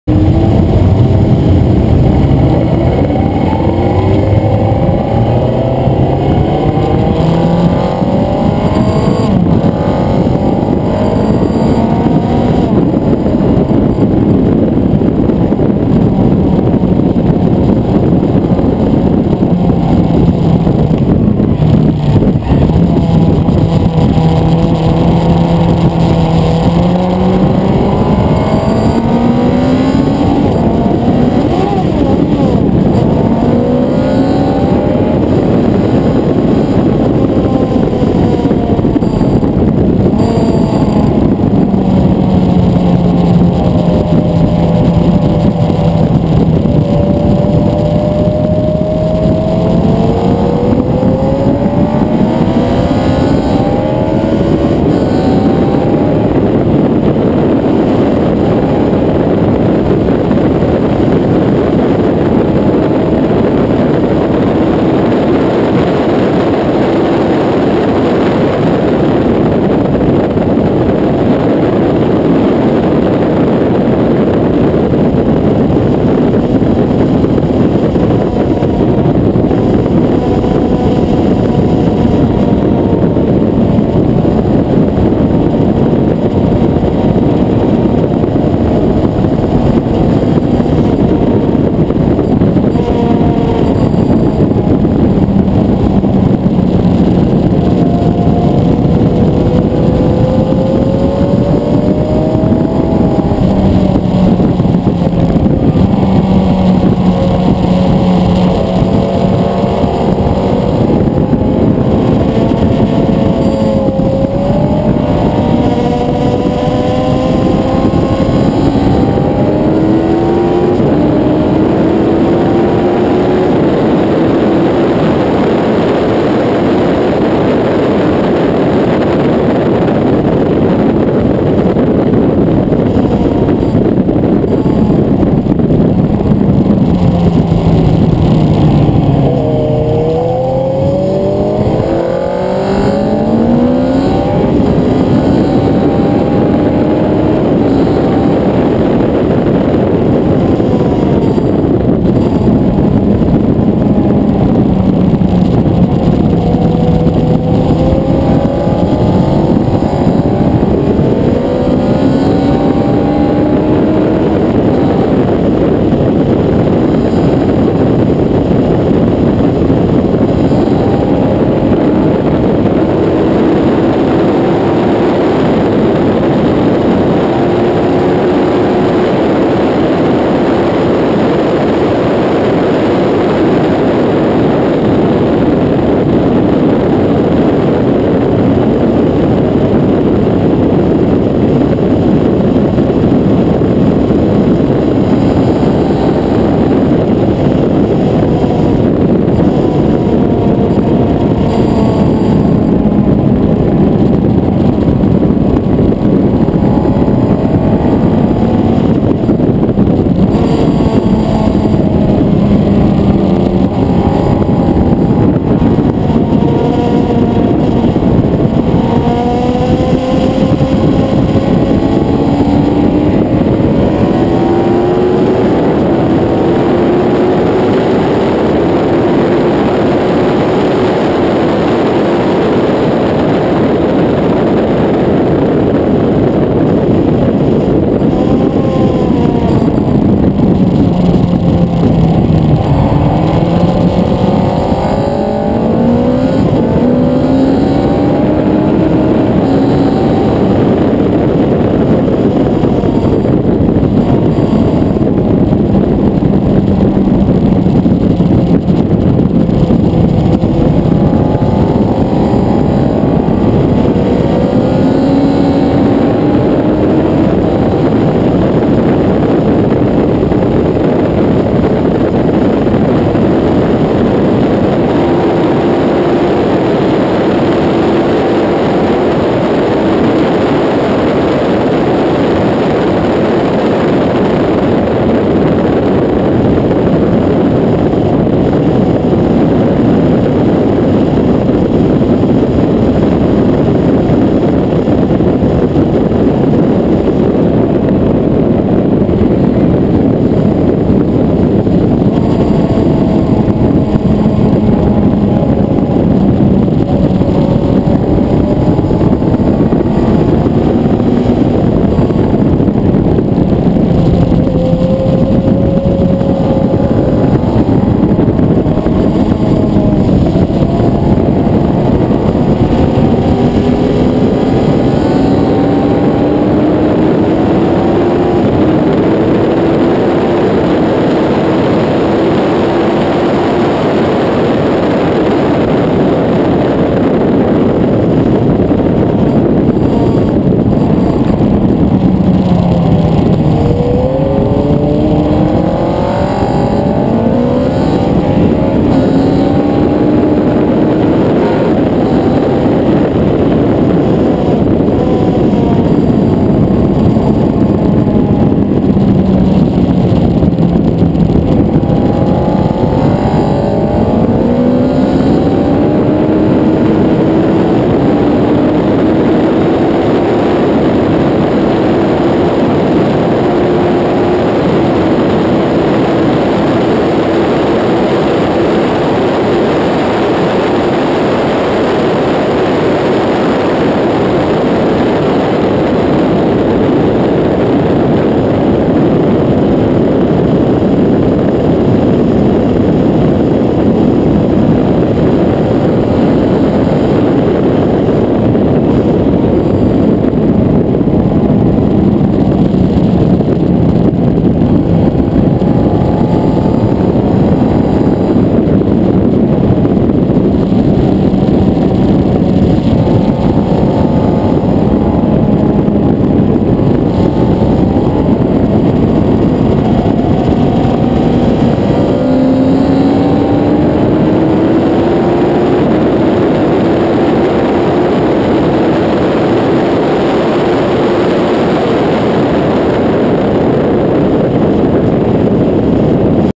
Salzburgring Freies Fahren Suzuki GSXR 1000
Freies Fahren Polizeimotorsportverein Tirol am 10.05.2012